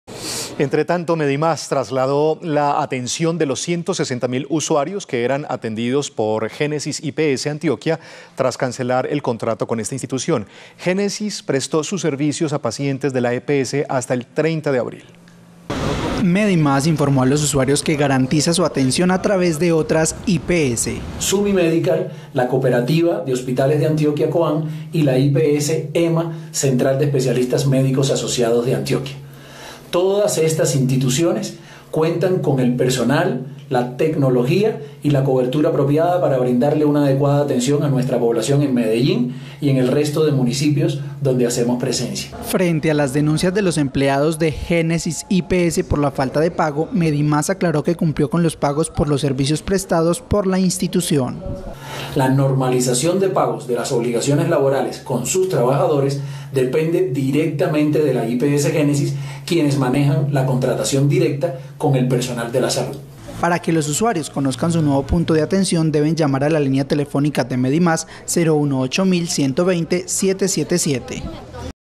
Medimás traslada servicios médicos de 160 mil usuarios
Medimás trasladó la atención de los 160 mil usuarios que eran atendidos por Génesis IPS Antioquia tras cancelar el contrato con esta institución. Génesis prestó sus servicios a pacientes de la EPS hasta el 30 de abril.